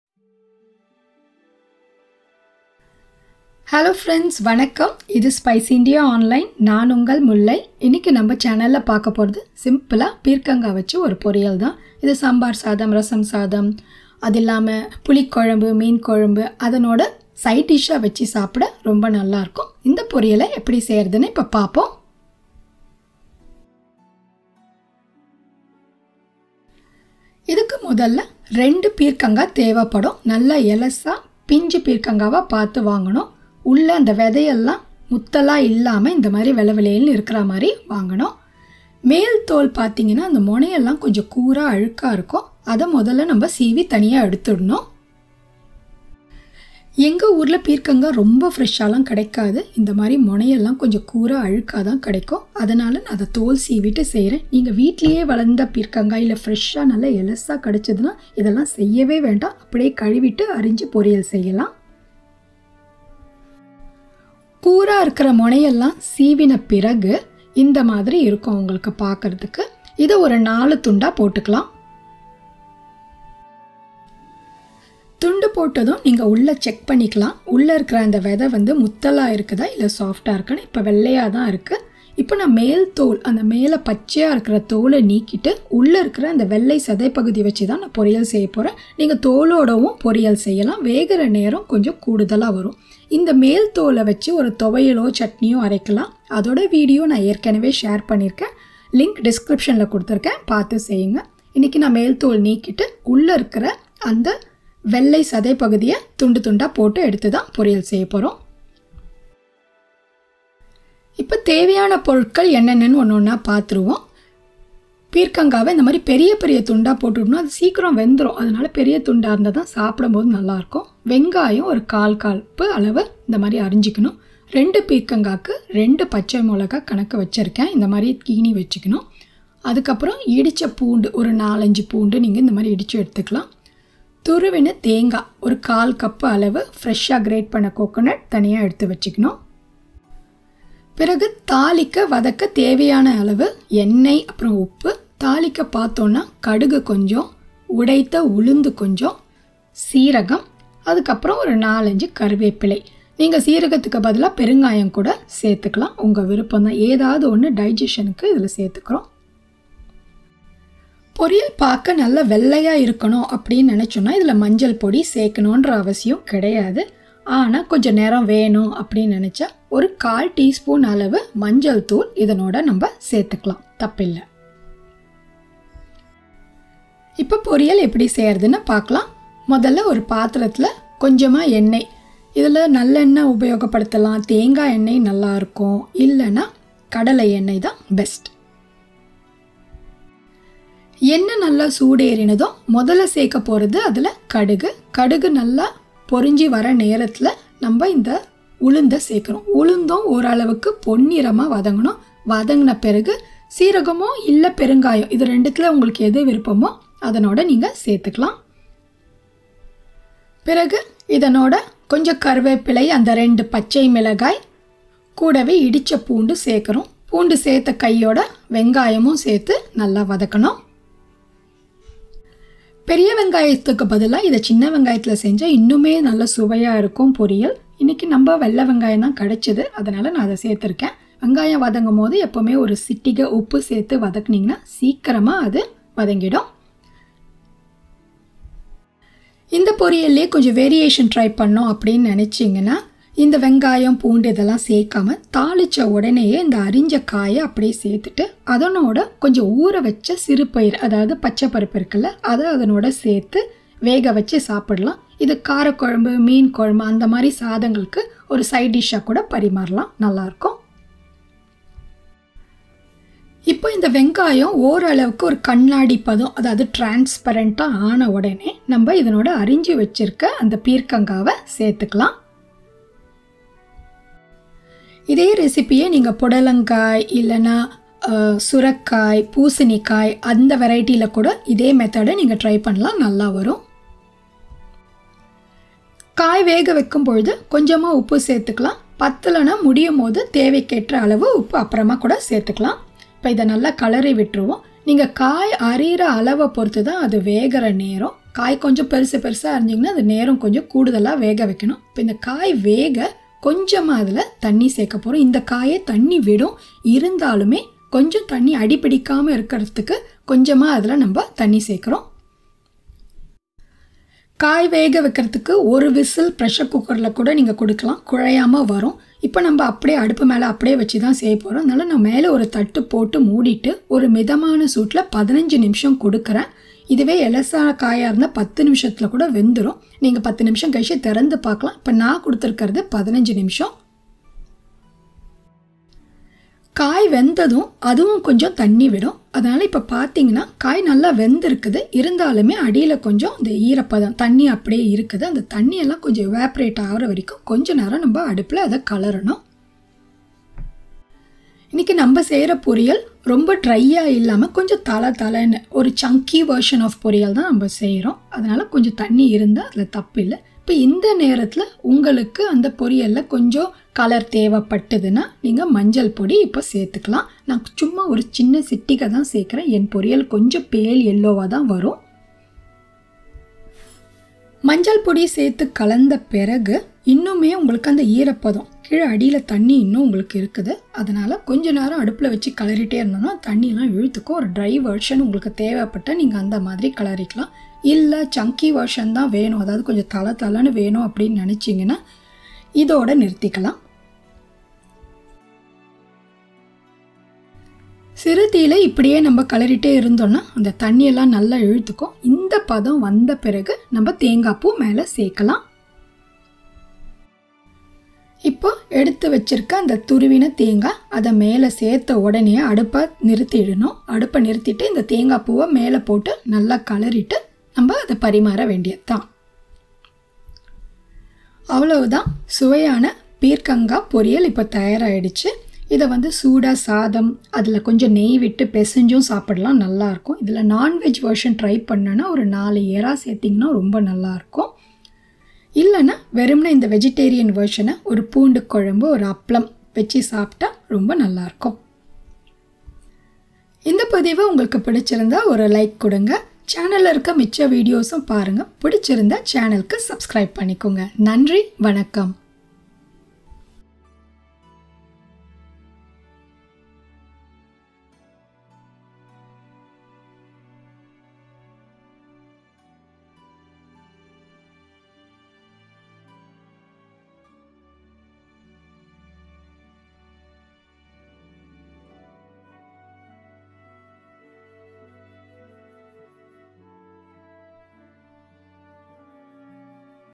Peerkangai Poriyal in Tamil | பீர்க்கங்காய் பொரியல் செய்வது எப்படி | Ridge Gourd Stir-Fry in Tamil
Peerkangai Poriyal Recipe - simple south indian style stir fry made with Ridge gourd.
Peerkangai Poriyal Recipe in Tamil | How to make Peerkangai Poriyal | பீர்க்கங்காய் பொரியல் செய்வது எப்படி